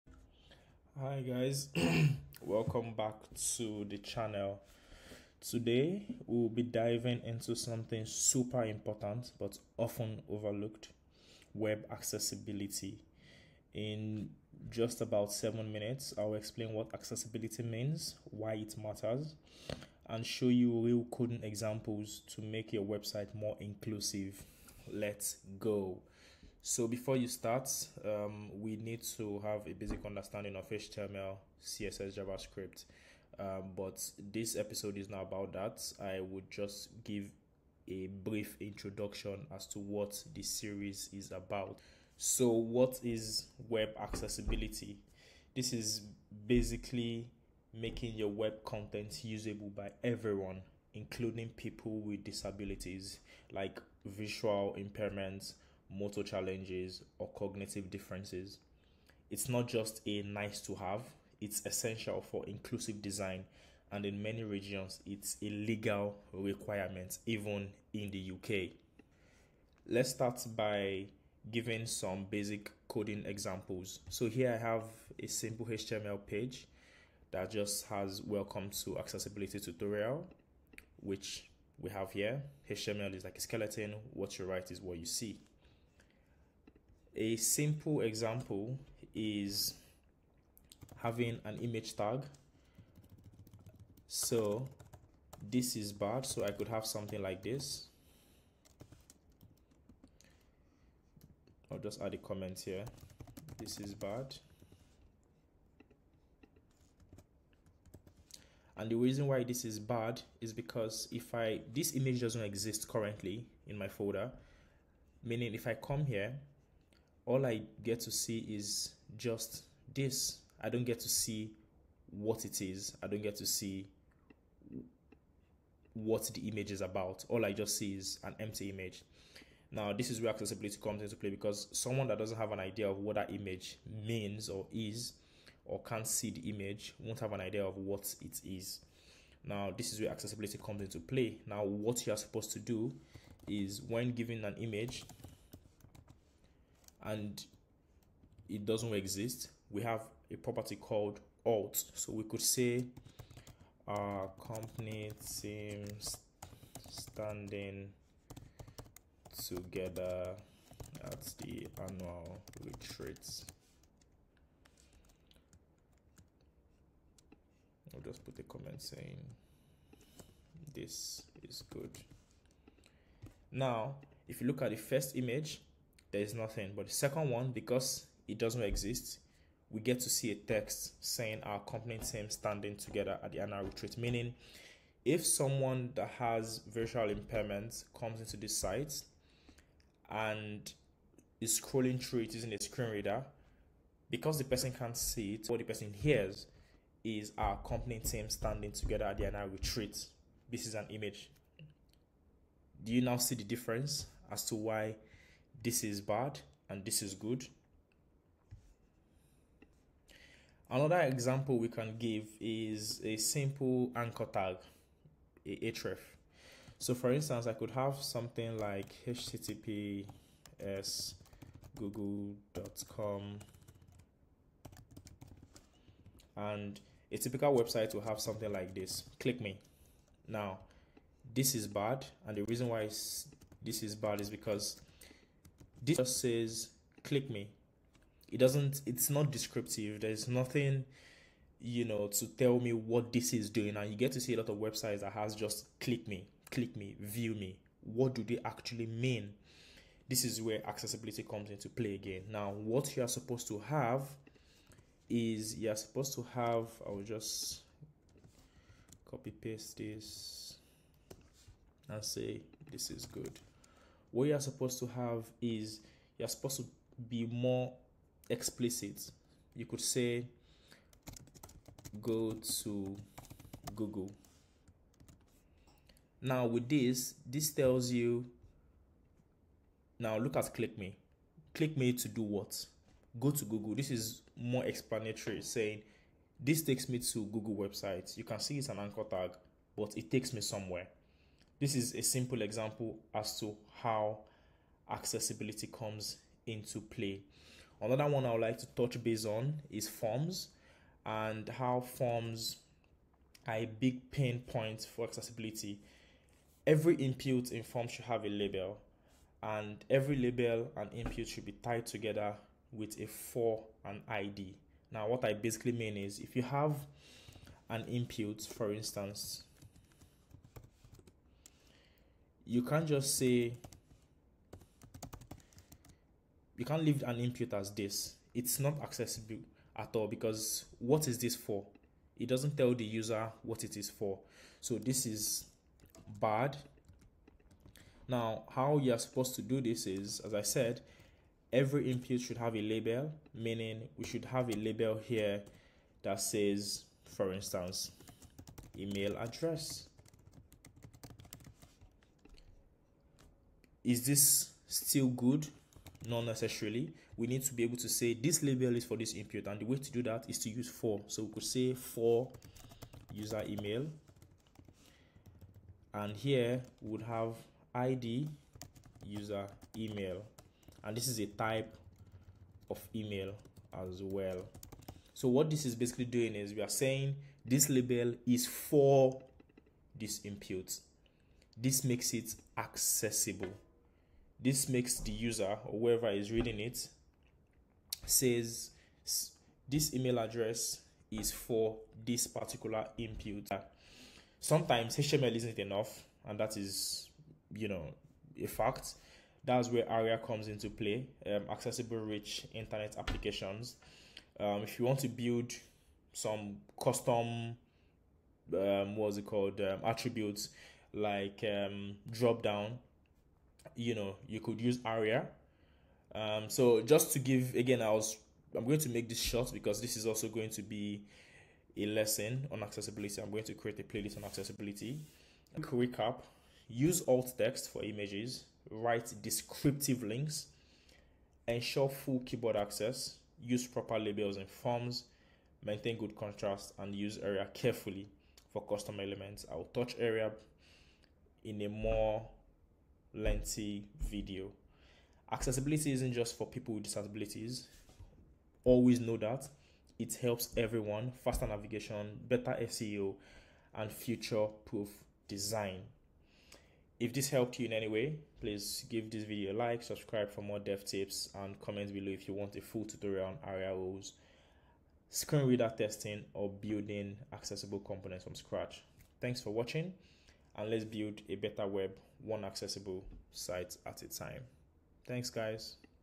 Introduction to Web Accessibility
This is an introduction to web accessibility and its importance in modern day web development.